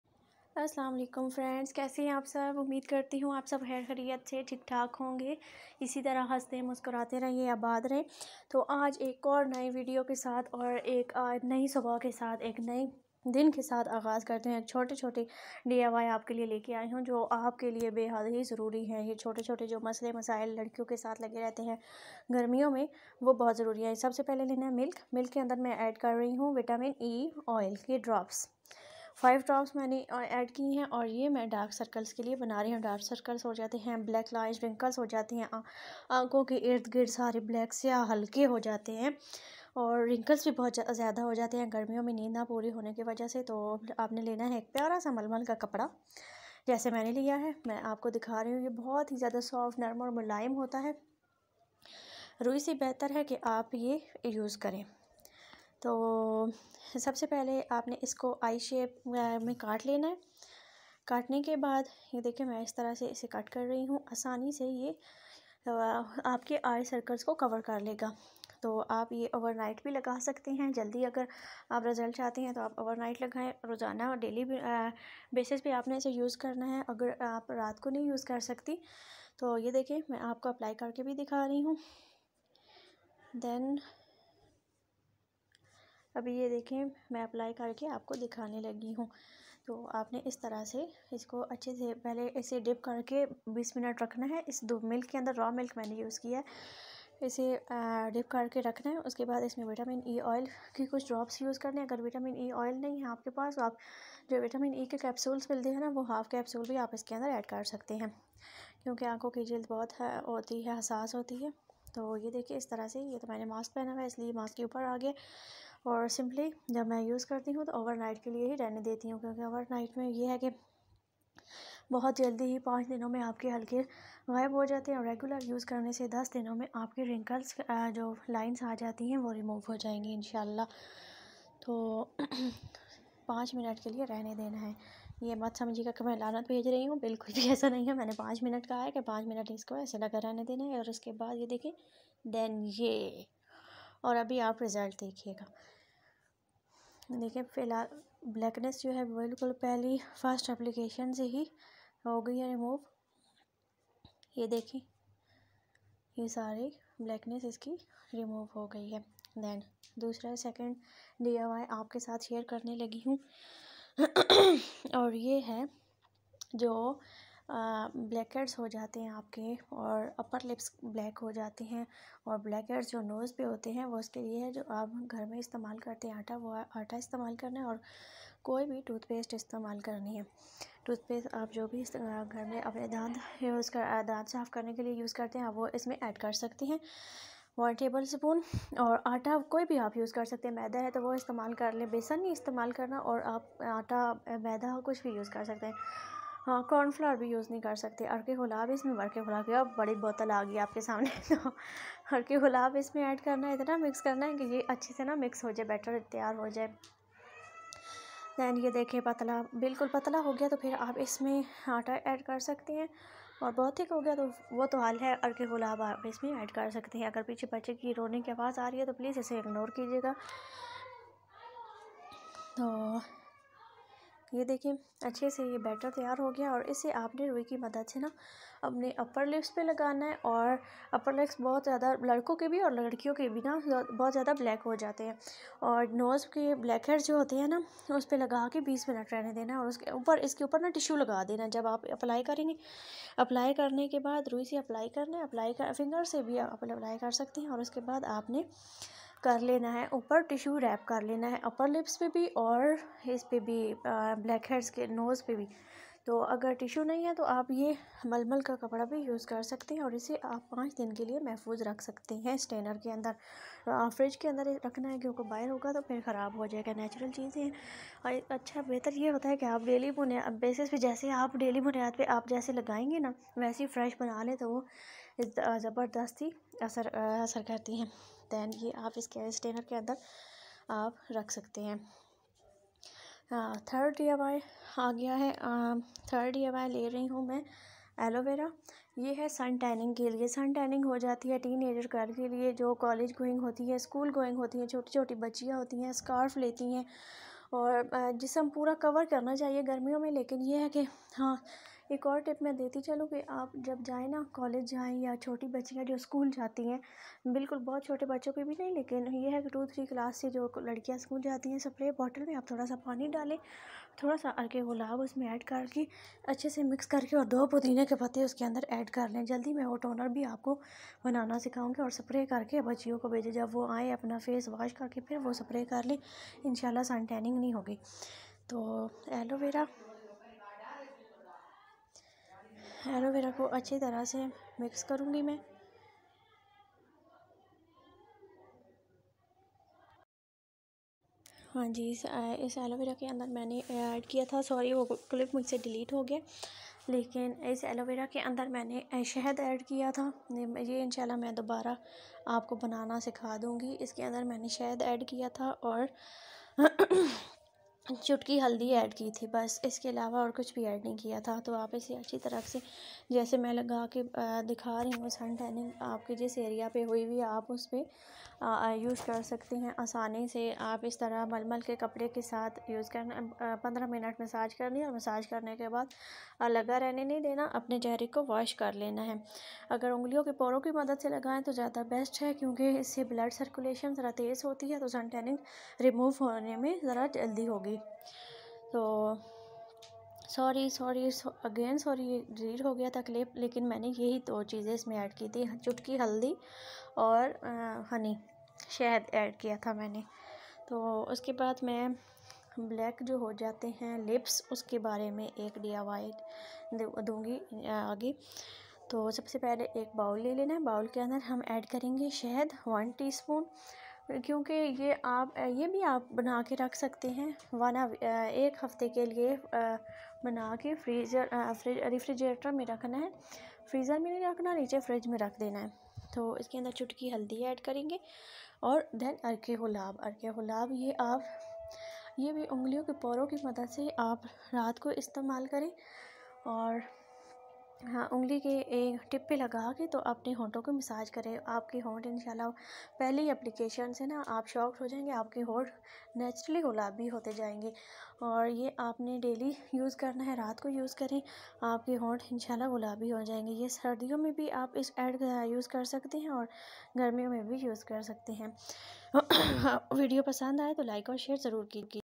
DIY for dark circles,easy remedy for suntan, diy for blackheads,dark lips treatment, natural remedys
in this video I can show you diys for dark circles under eyes wrinkles , DIY for blackheads and dark upar lips, simple treatment for suntan, diy for lips all girls problem are solve in this video,so watch like and share